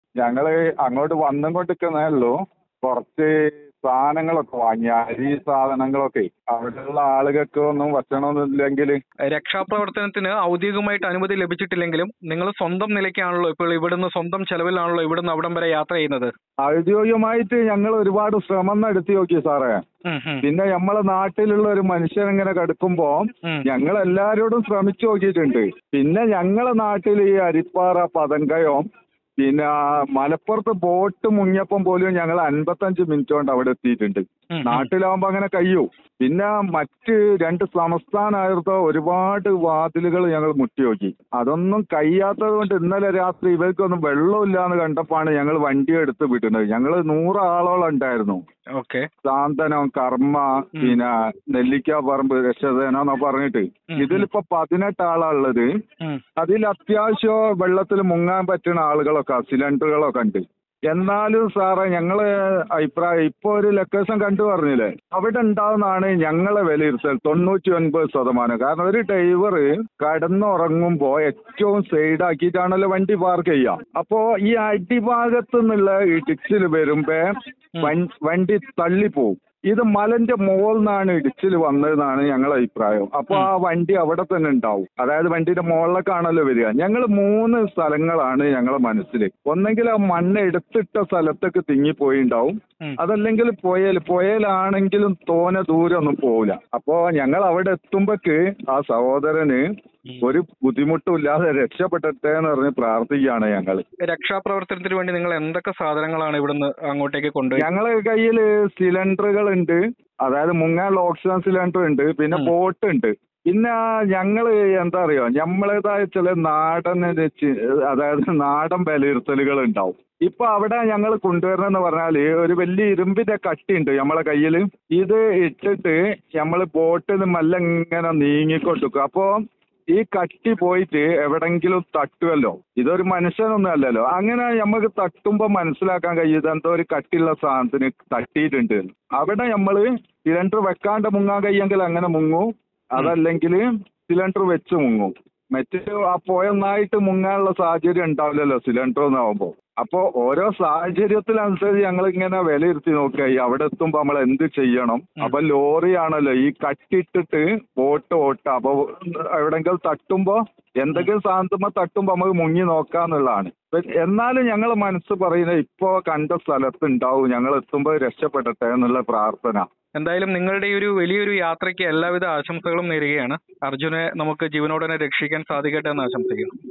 'നമ്മുടെ നാട്ടിലുള്ള ഒരു മനുഷ്യൻ മണ്ണിനടിയിൽ കിടക്കുമ്പോൾ നമ്മൾ പോകണ്ടേ?'; പ്രതികരിച്ച് മലയാളി സംഘം
Lorry Driver Arjun Rescue Mission: A Rescue team fror Kozhikode departed to Shiroor, Karnataka | 'നമ്മുടെ നാട്ടിലുള്ള ഒരു മനുഷ്യൻ മണ്ണിനടിയിൽ കിടക്കുമ്പോൾ നമ്മൾ പോകണ്ടേ?'; Oneindiaയോട് പ്രതികരിച്ച് മലയാളി സംഘം

~PR.272~ED.22~HT.24~